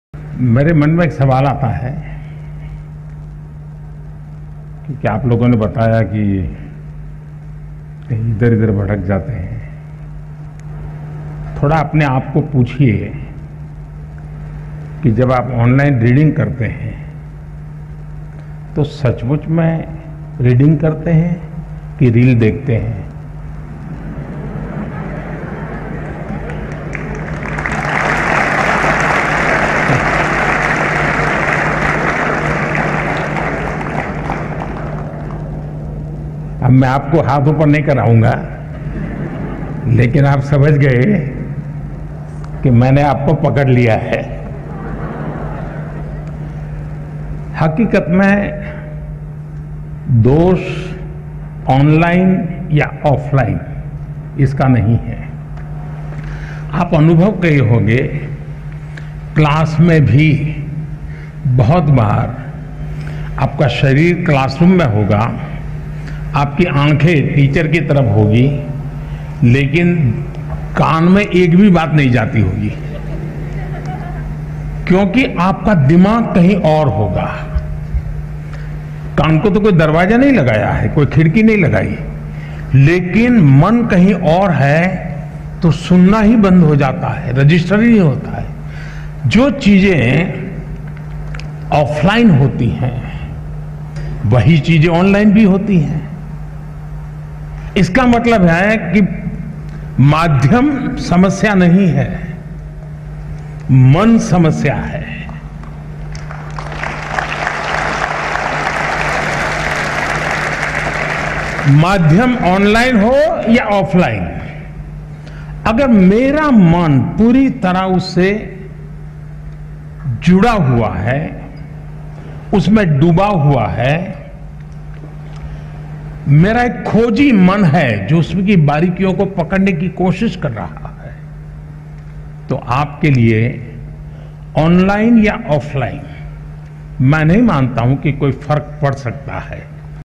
Message regarding concentration during offline and online mode studies by Honourable Prime Minister
पढ़ाई ऑनलाइन हो या ऑफलाइन, एकाग्रता जरूरी है - Message by Honourable Prime Minister Shri. Narendra Modi in Pariksha Pe Charcha 5.0.